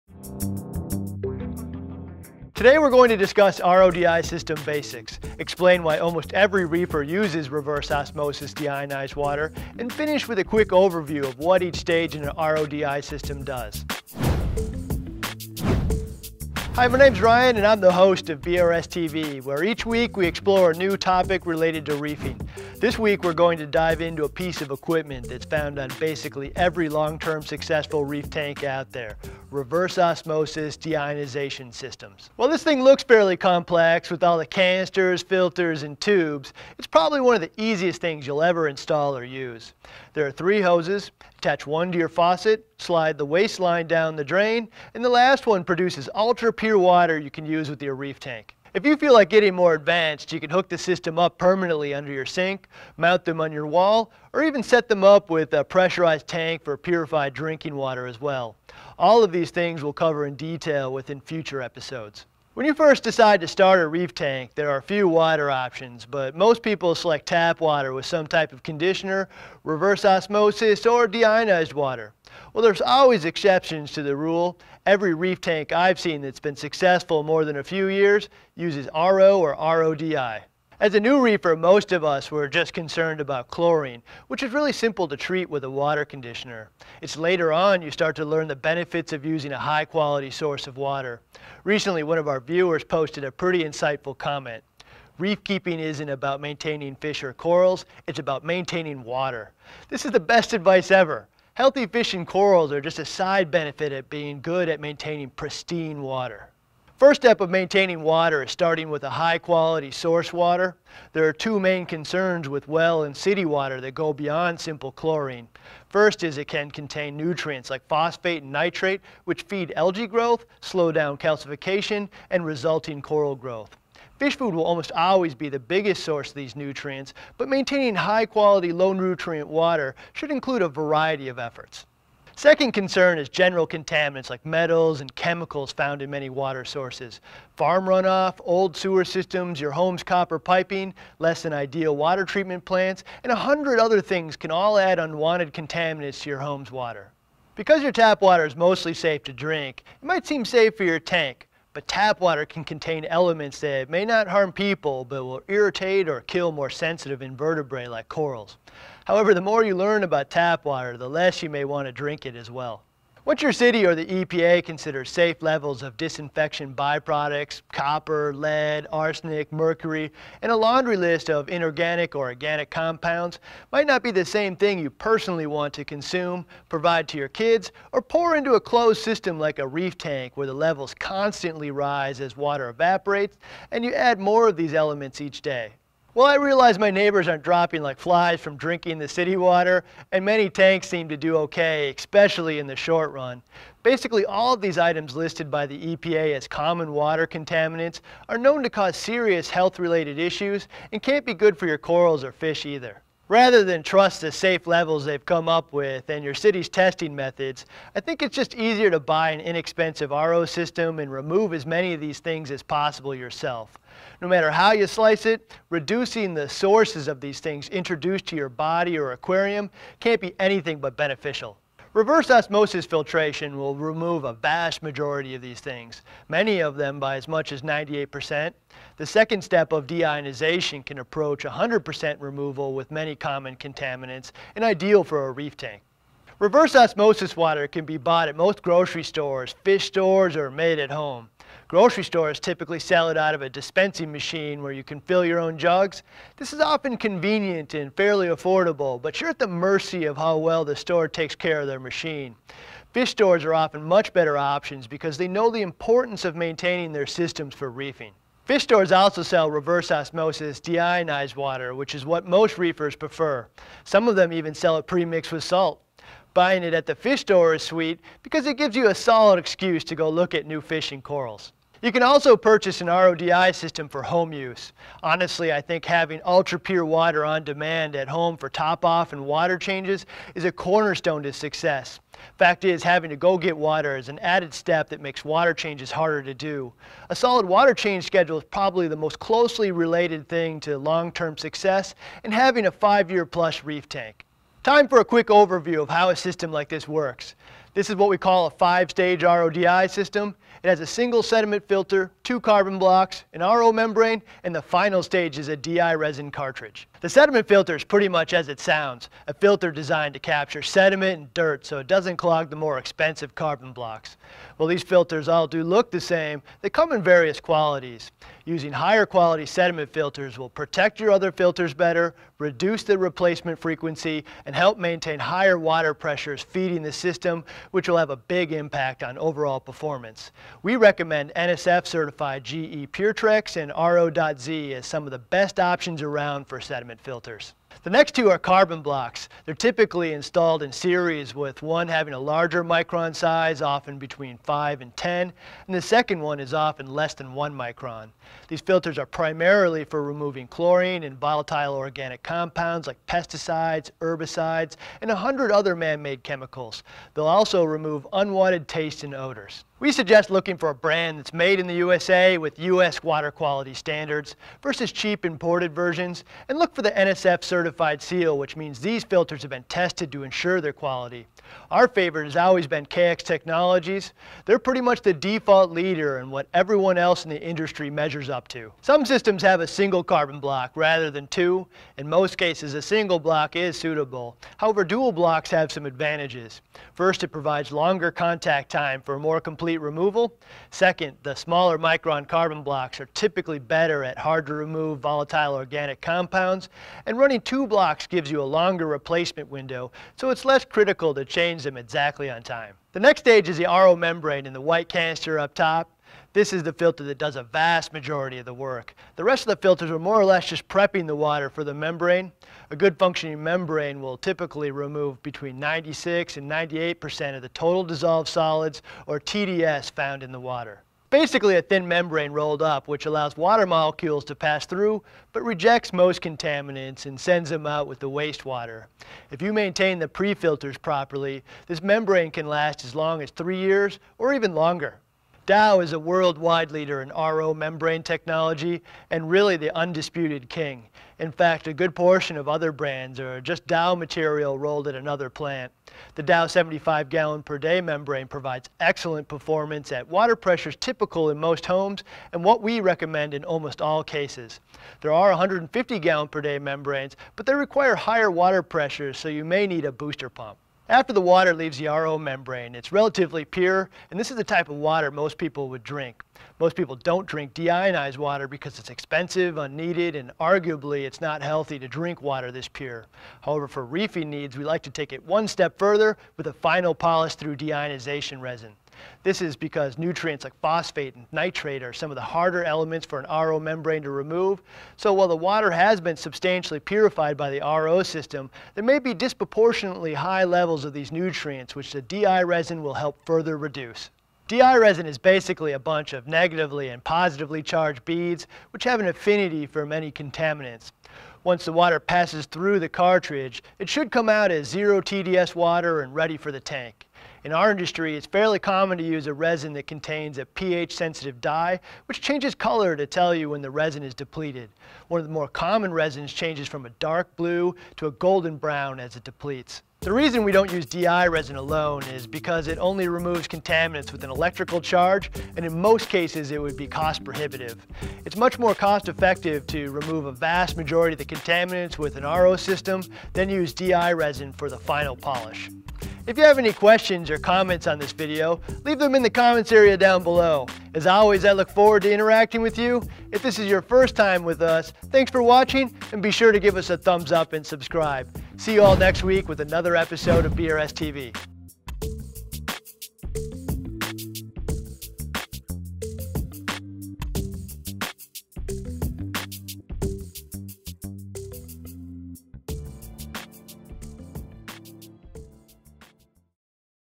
Reverse Osmosis System Basics - EP 1: Reverse Osmosis Systems and Your Reef Tank
Today on BRStv, we discuss the basics of a reverse osmosis/deionization water purification system.  Learn why so many reefers find this an essential part of their saltwater set up and how it can improve the health of your corals and fish.  Finally, we'll explain what each state of the ro/di system does.

RO/DI systems can look fairly complicated but they are actually simple to install.  There are three hoses -- one attaches to your faucet or water source, one is for your waste water and finally, one is for the ultra pure water you can use for your reef tank.  Reverse Osmosis systems are great additions to a reef aquarium set up because it eliminates the need to haul water from the fish store or purified water from the grocery store -- many times saving time and money.